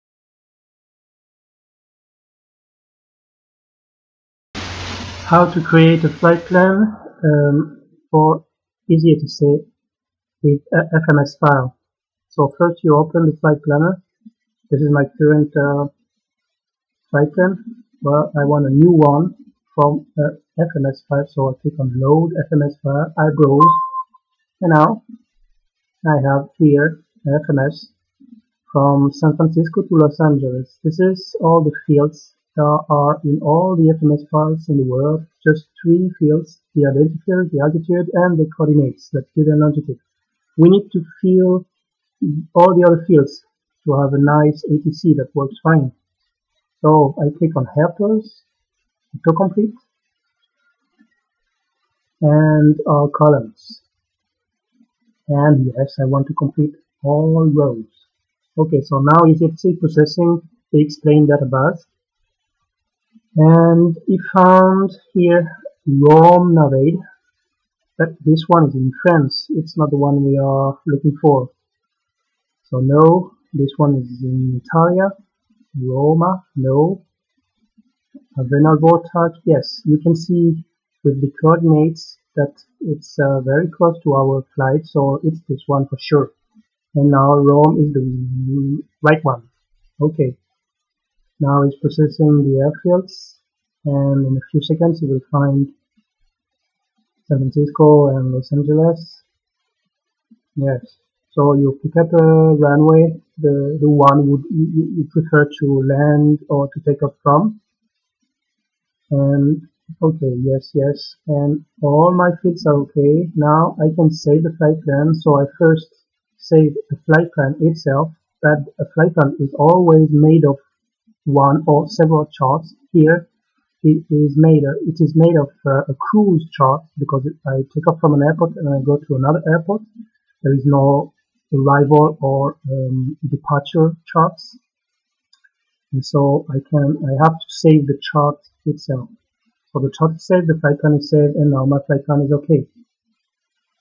EasyATC - How to make a flight plan from a FMS file?
Tutorial video where you can see how to make a flight plan for EasyATC 3.0 by importing a FMS file